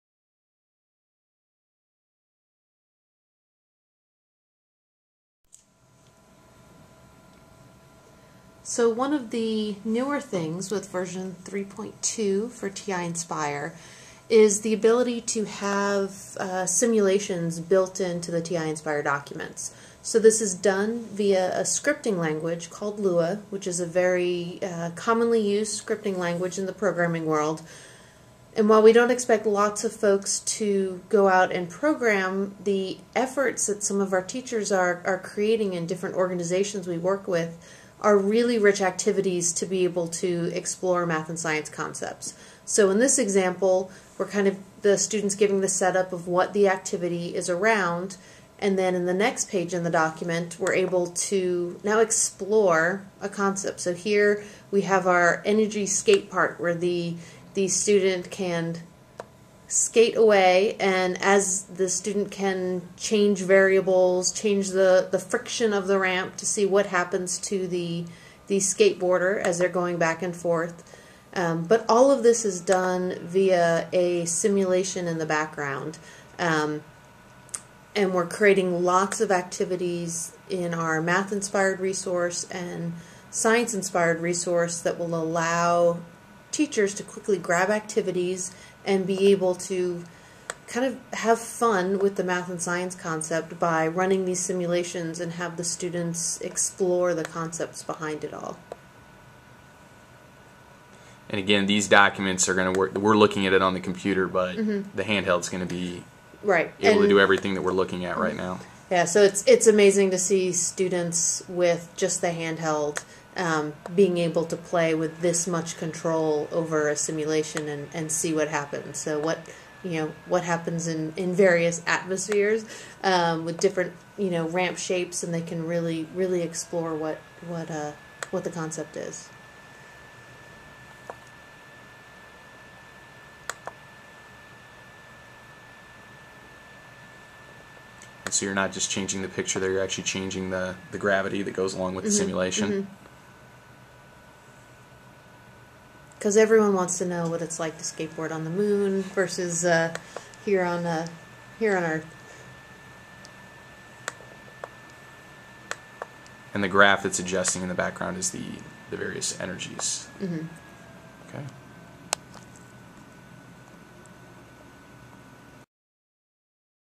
TI-Nspire 3.2 Lua Scripting
TI-Nspire OS 3.2 has Lua scripting support. At the T3 conference in Chicago, the folks at Texas Instruments gave me a quick demonstration of what it can do. Sorry for the shaky camera at the start! TI-Nspire OS 3.2 will be released in late May 2012.